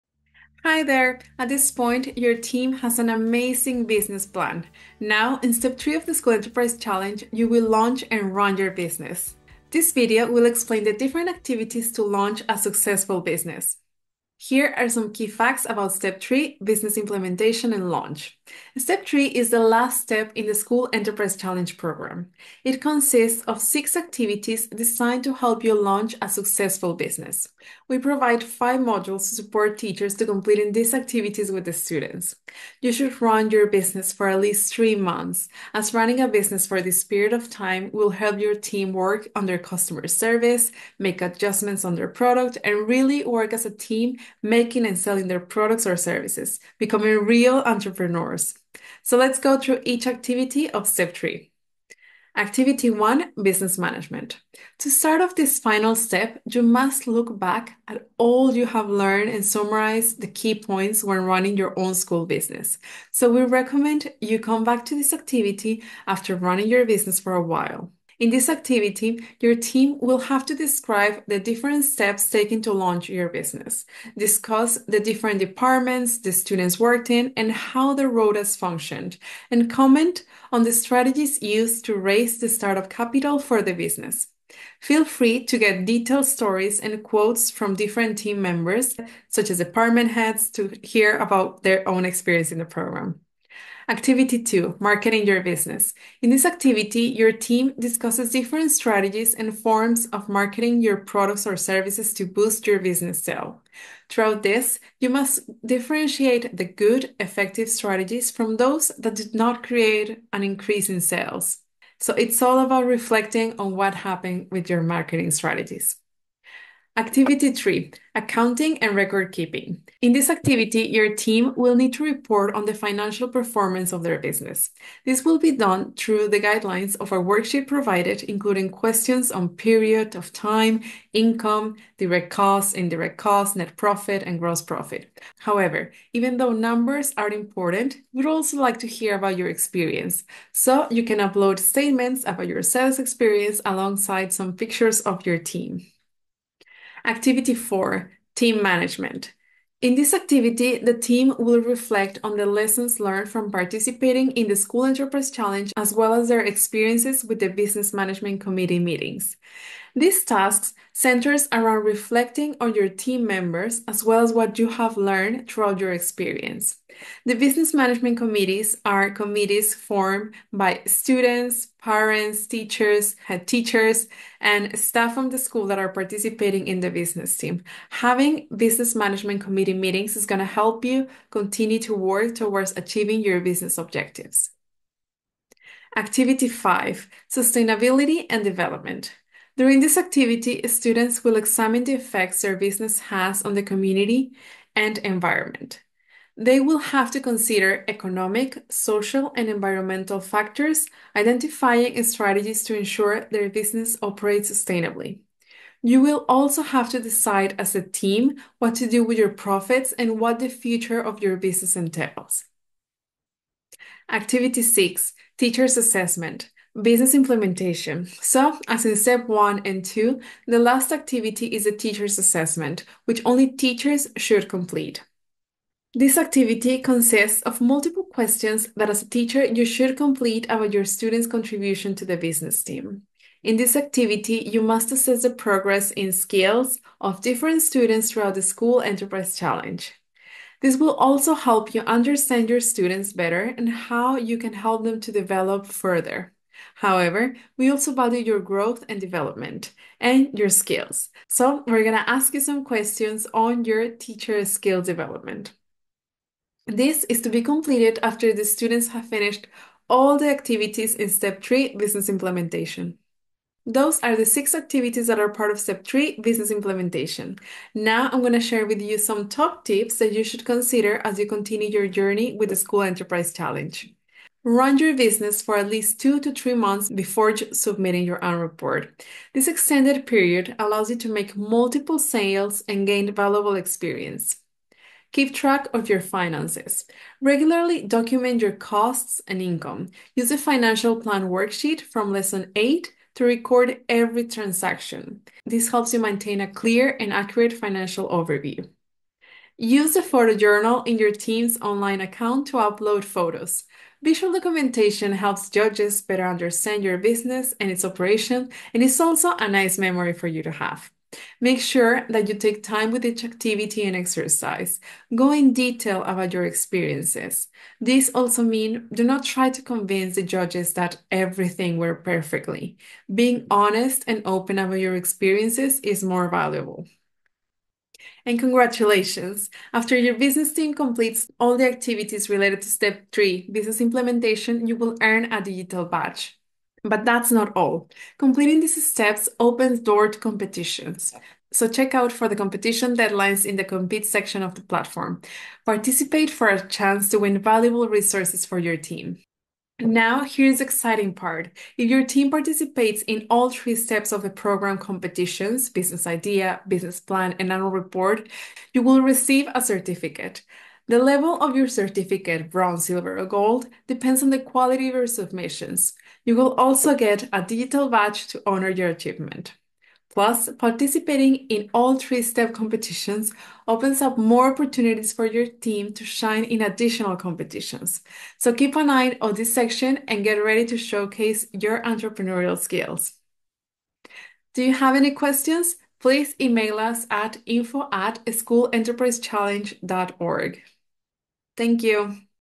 Step 3: Business Implementation - School Enterprise Challenge 2024 on ElevatED
Welcome to Step 3 of the School Enterprise Challenge! In this video, we’ll guide you through each activity needed to complete the last step of the programme:

- Activity 1: Business Management (0:52) - Your business team will reflect on the steps taken to launch your business.
- Activity 2: Marketing Your Business (1:33) - Your students will analyse and discuss the effectiveness of the marketing strategies used to promote your products or services and boost sales.
- Activity 3: Accounting and Record Keeping (1:56) - Your students will report on your business’s financial performance, including income, costs, and profits, and share your experiences along with visual documentation.
- Activity 4: Team Management (2:27) - Have your students reflect on the key lessons learned from participating in the School Enterprise Challenge and the insights gained from Business Management Committee meetings.
- Activity 5: Sustainability and Development (3:05) - Have your students examine the impact of your business on the community and environment, and develop strategies for sustainable operations and future planning.
- Activity 6: Teacher’s Assessment (3:34) - Complete an assessment evaluating students' contributions and skill development throughout the School Enterprise Challenge, and reflect on your own growth as a teacher.

Congratulations! By completing these activities, you will finish the School Enterprise Challenge and unlock opportunities to enter competitions.

Make sure to complete each activity on our ElevatED App to participate in the programme.